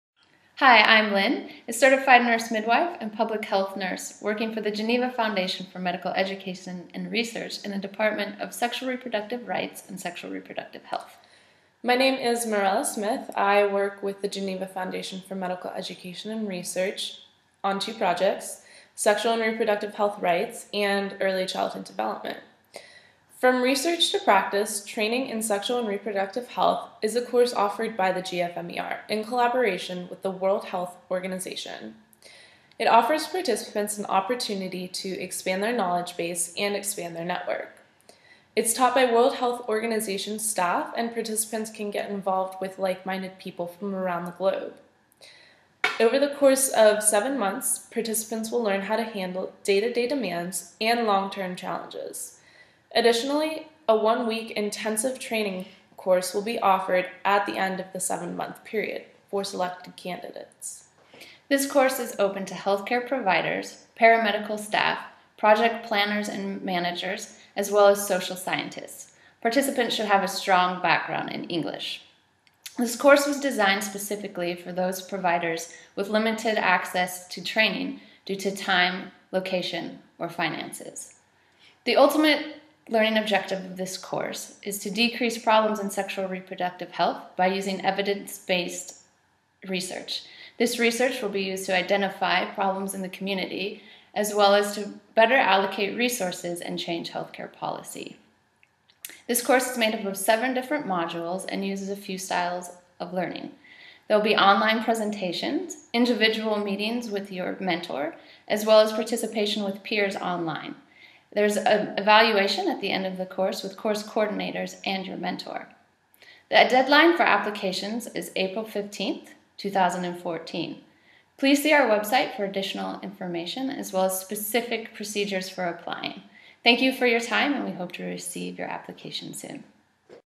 GFMER Training Course in Sexual and Reproductive Health Research 2014
The Geneva Foundation for Medical Education and Research (GFMER) in collaboration with the World Health Organization is proud to offer our online training course: "From Research to Practice: Training Course in Sexual and Reproductive Health."  The course will take place over a period of seven months and is catered towards health professionals with limited access to financial or educational resources and time. Our main objective is to provide health professionals with valuable knowledge and skills that can be applied to a variety of challenges in the sexual and reproductive health field. It is a great opportunity for those looking to create a strong network, share information, and increase their knowledge and skill capacity. An application is required and must be completed by April 15th, 2014. For more details, please see our brochure